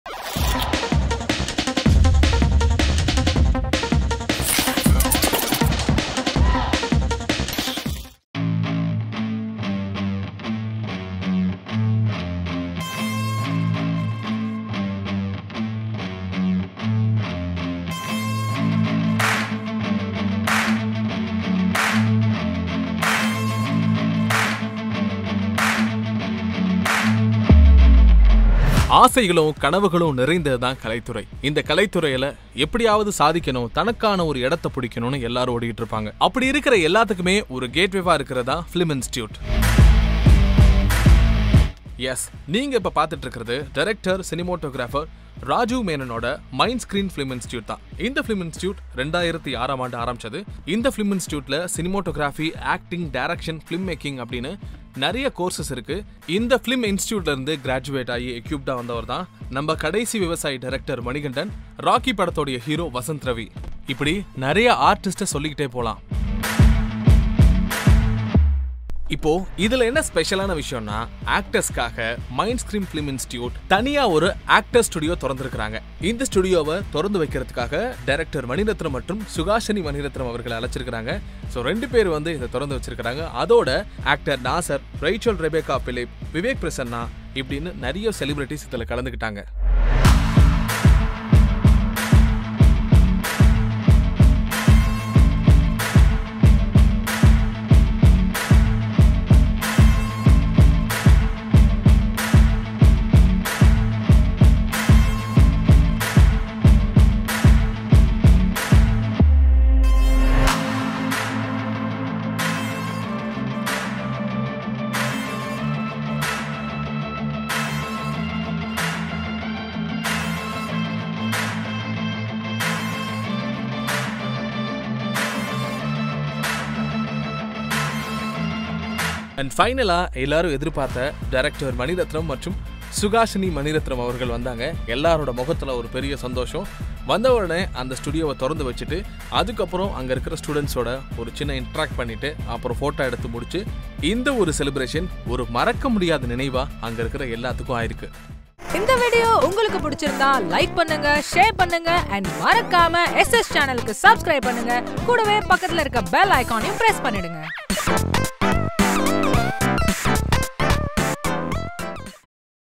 Mindscreen Actors Studio - Inauguration - Rajiv Menon, Mani Ratnam, Suhasini Maniratnam, Nasser
MINDSCREEN FILM INSTITUTE (MFI) was founded by Director, Cinematographer Rajiv Menon in 2006 as school of cinematography and expanded to filmmaking direction and acting. More than 800 students have graduated in cinematography, filmmaking and acting with 350 feature films in all languages in a span of a decade. To name a few, alumni Manikandan - Director of Kadaisi Vivasayi, Vivek Prasanna, Ananth nag & Vasant Ravi (Rocky). The list never ends here with more and more successful alumni’s.

Now MINDSCREEN FILM INSTITUTE (MFI) has a separate actor’s studio to train and bring out best talents to the film industry and the same was inaugurated by Director Mani Ratnam & Suhasini Maniratnam. The function was graced by Actor Nasser, Raichal Rabecca Philip, Vivek Prasanna and other film fraternities from the industry.